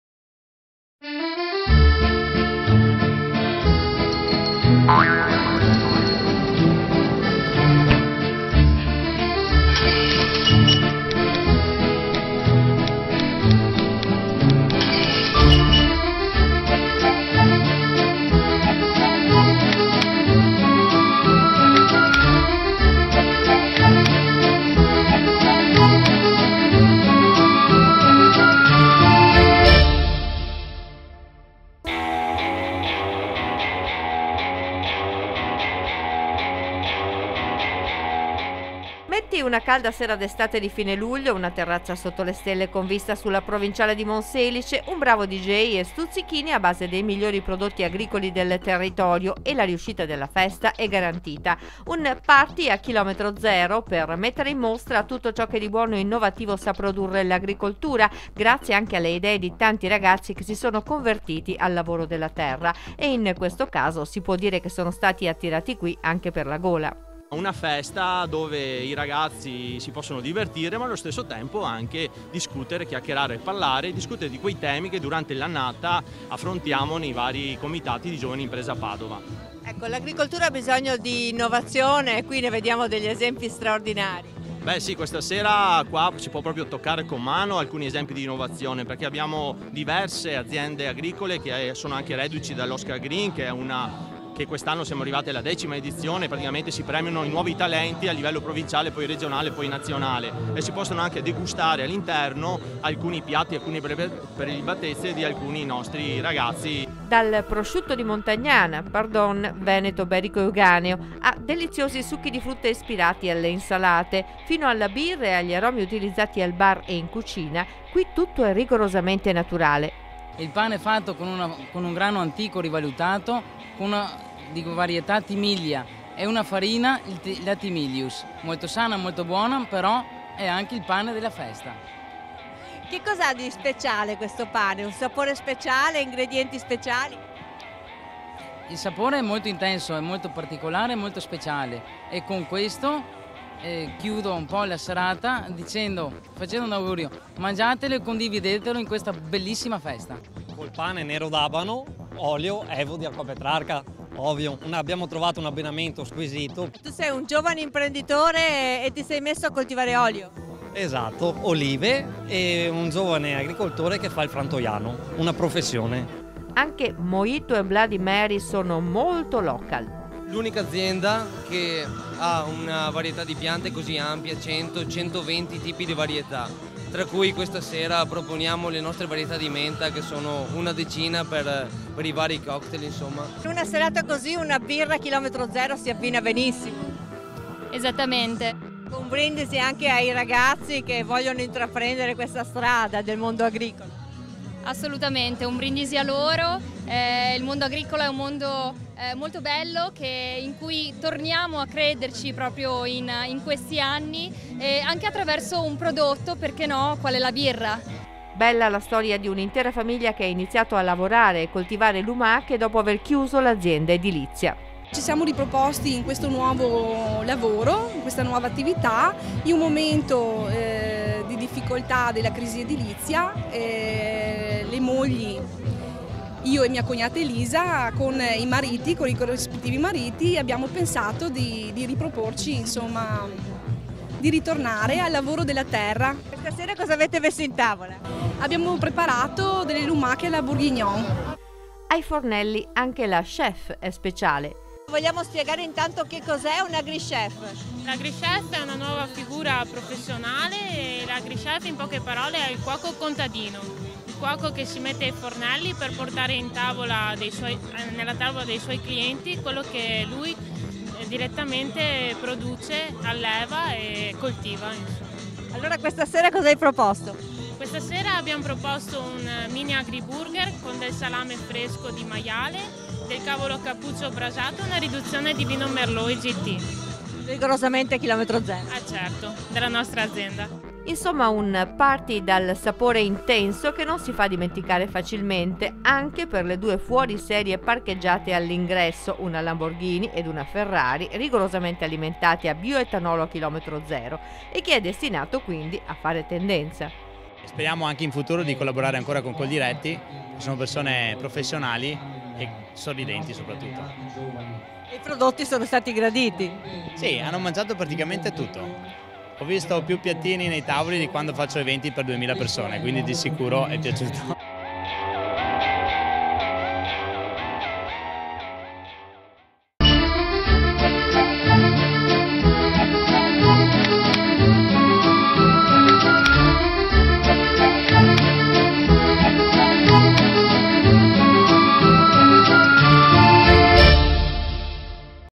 Agrisummer, festa d'estate
A Monselice (Pd) i giovani della Coldiretti di Padova hanno organizzato un party a Km 0 per promuovere i prodotti delle loro imprese agricole.